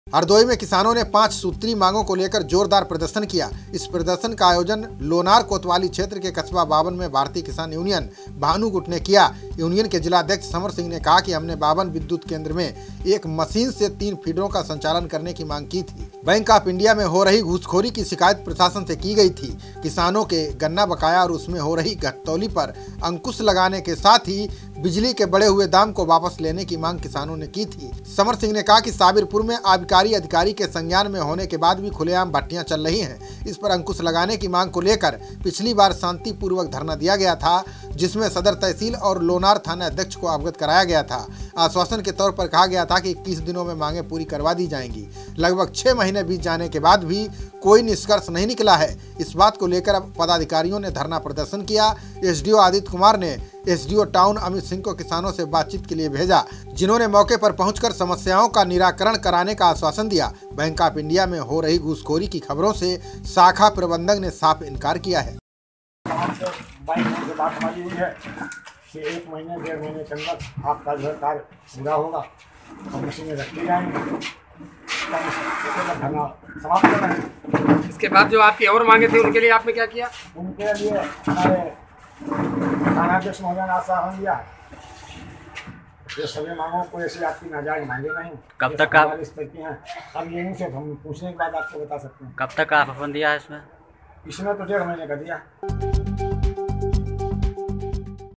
हरदोई के किसानों ने कैसे खोली प्रशासन की पोल...
किसानों ने किया जोरदार प्रदर्शन
भारतीय किसान यूनियन ने किया नेतृत्व
समर सिंह ने रखी कई मांगे
बैंक आॅफ इंडिया में घूसखोरी का उठाया मामला